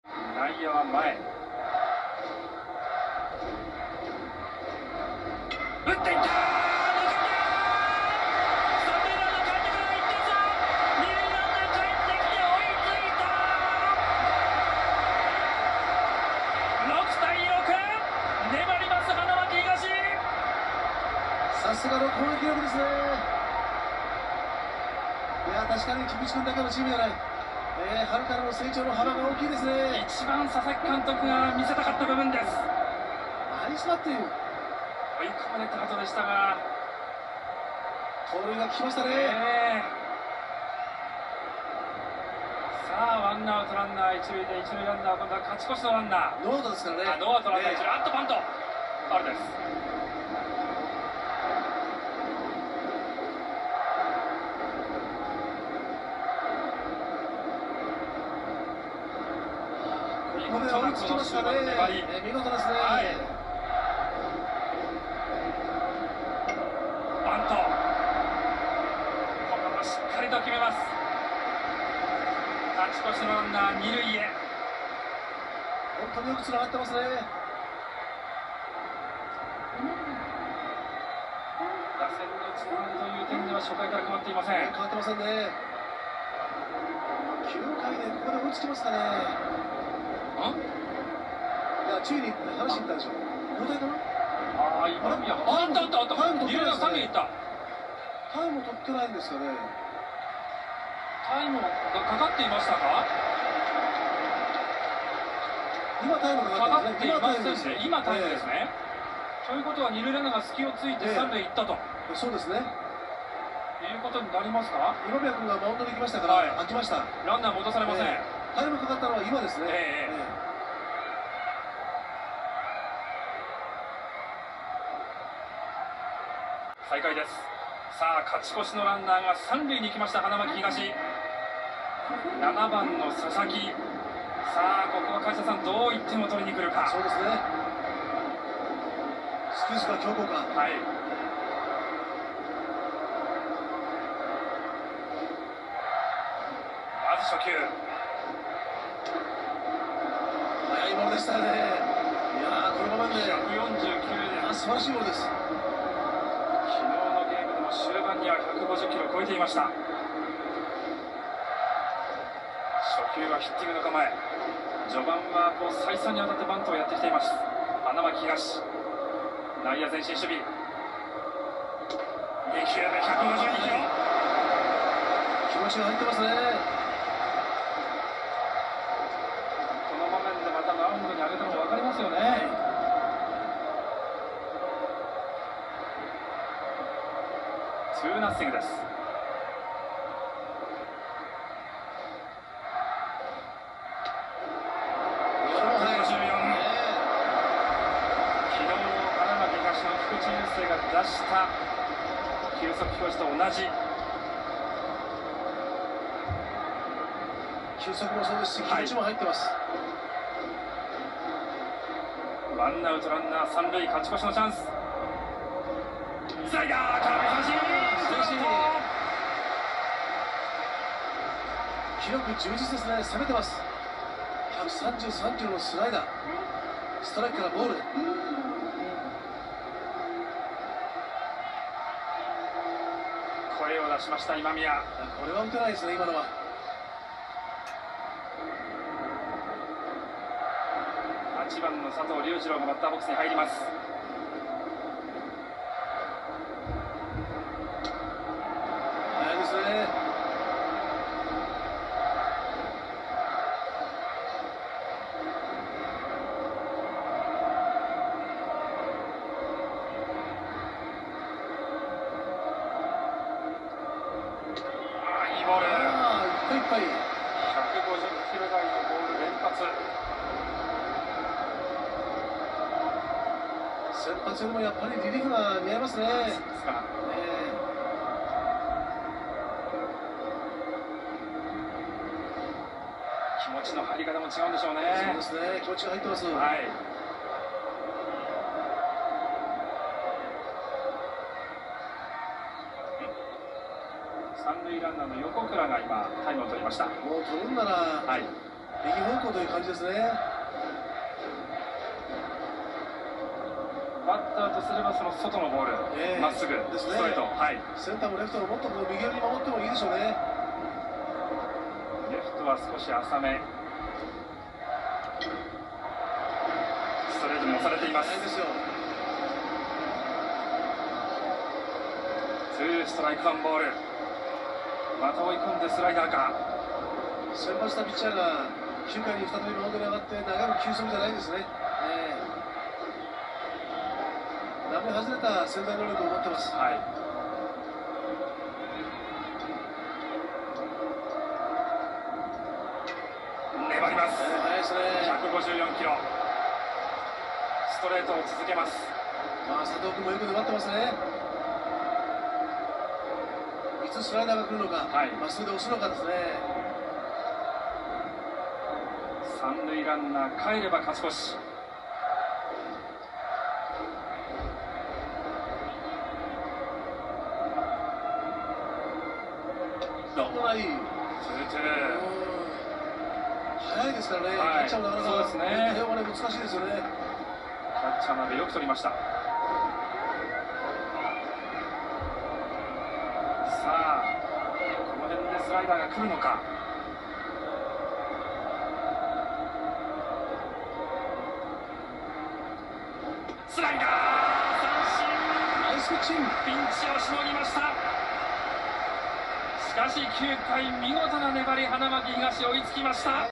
【覚 醒】投手・今宮健太 伝説の１０球！【明豊×花巻東】
現在、プロ野球の福岡ソフトバンクホークスで遊撃手で活躍中の今宮健太が明豊高校時代に投手として最速の154km/hを計測したときの様子です。一打同点の場面でのリリーフ登板は一部で伝説の１０球と呼ばれているらしいです。